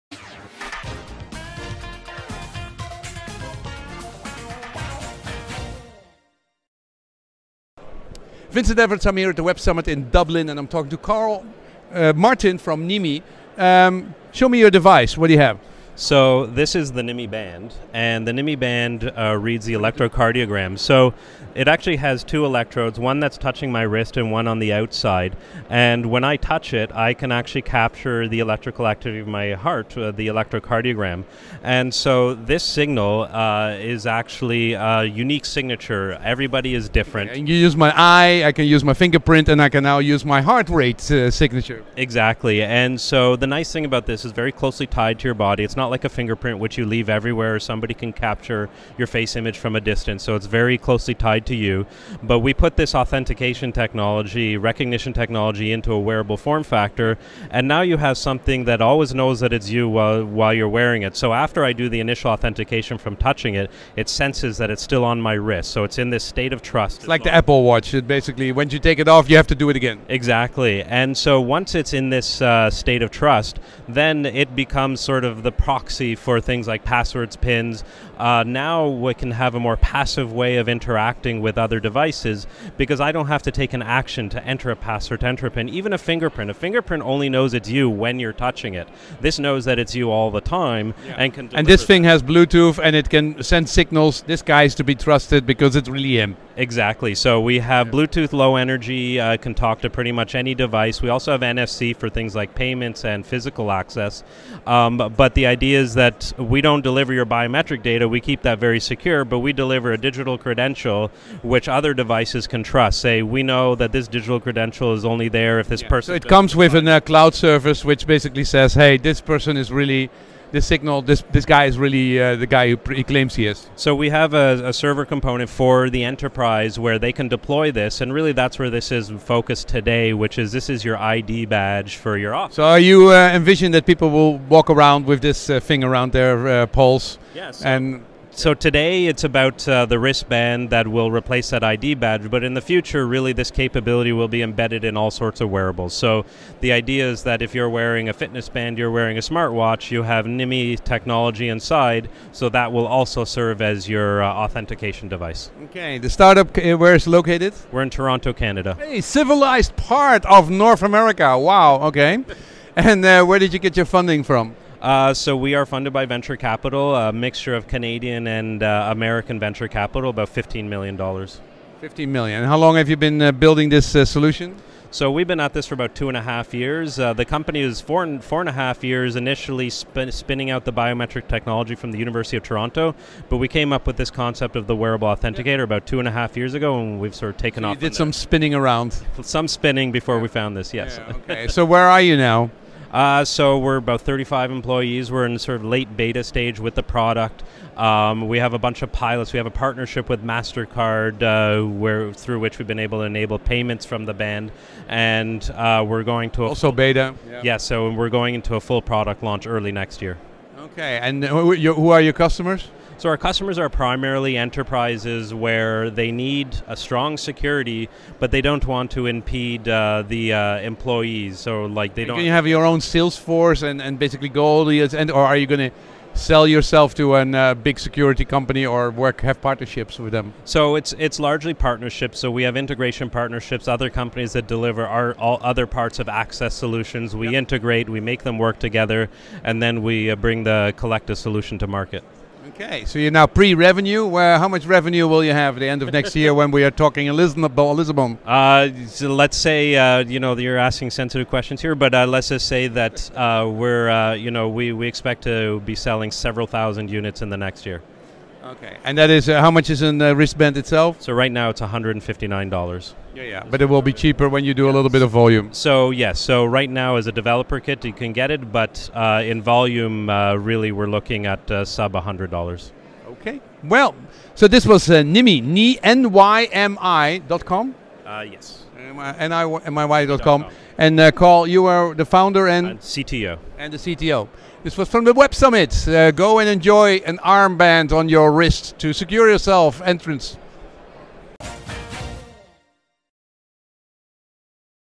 Karl Martin founder $15m funded NYML.com which takes heartra
Karl Martin founder $15m funded NYML.com which takes heartrate as biometric security checkin websummit2015 is published on event Baldopinions